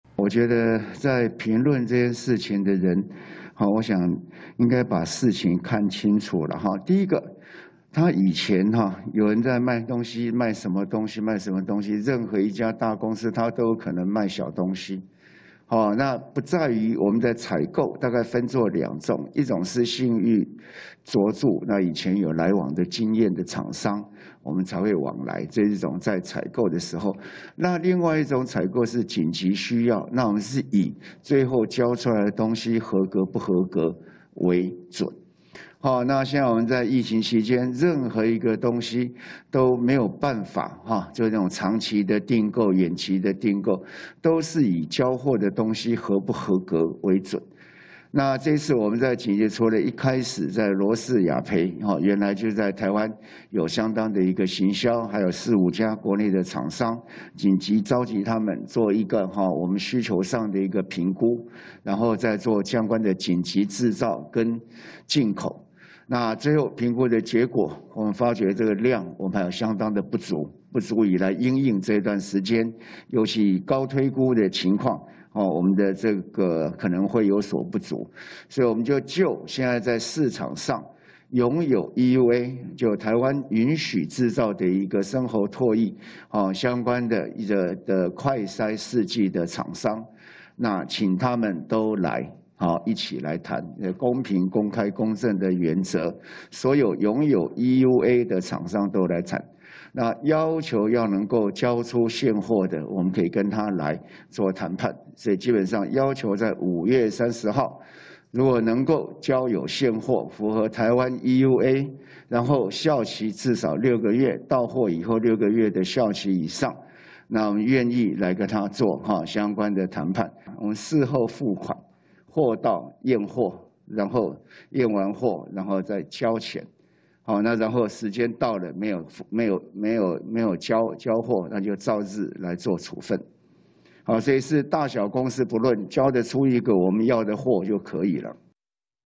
小吃店變身"接16億快篩訂單" 陳時中:貨到合格才付款│中視新聞 20220504
針對外界質疑，快篩試劑採購過程有利益輸送，對此，指揮官陳時中回應，任何一家大公司都可能賣小東西，採購在「緊急需要時，會以最後交出來東西合不合格為準」。